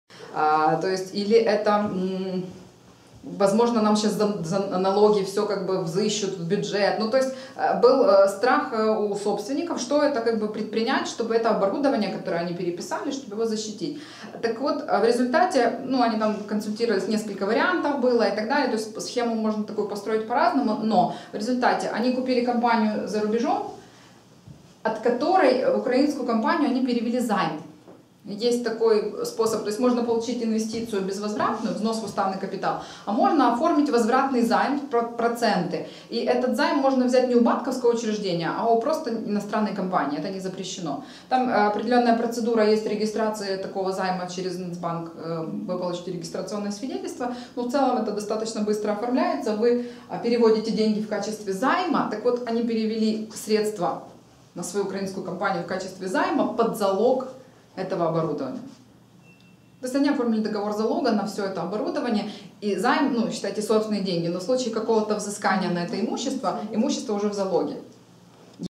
Защита имущества от взыскания путем передачи в залог иностранной компании
Александра Федак, практикум КЦПРБ "Налоговое планирование и оптимизация налогообложения"